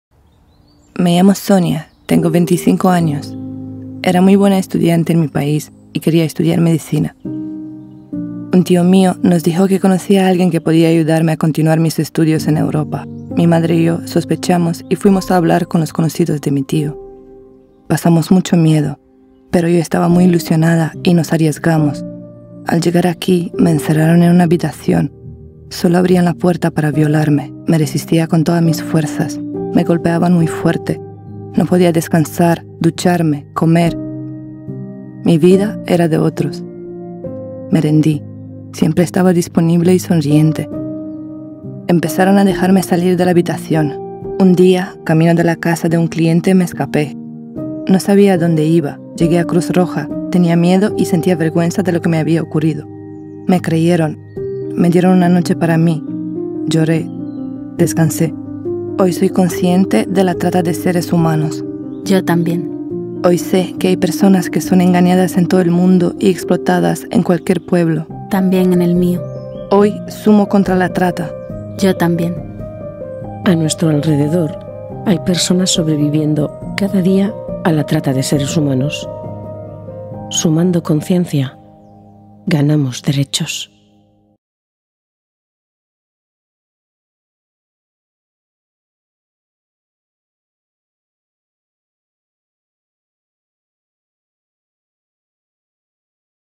Me llamo Sonia, tengo 25 años y soy superviviente de la trata | Inclusión Social
Sumando conciencia ganamos derechos. Esta es una historia basada en vidas reales que ha sido recreada por actrices y actores para preservar la seguridad e intimidad de quienes sobreviven a la Trata de Personas.